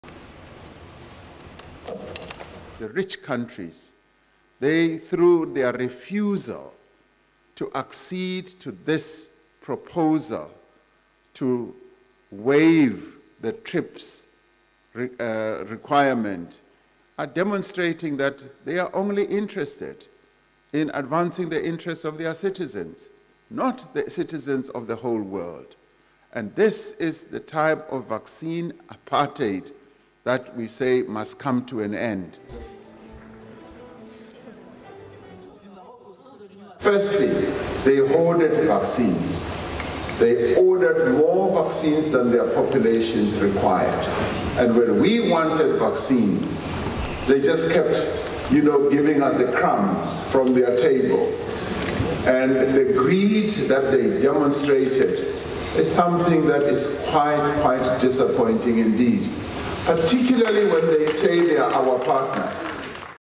Ramaphosa says rich nations fuel vaccine apartheid
South Africa's President Cyril Ramaphosa says he is disappointed by rich countries and their approach to vaccinating the world, describing how poorer nations have been 'left the crumbs'.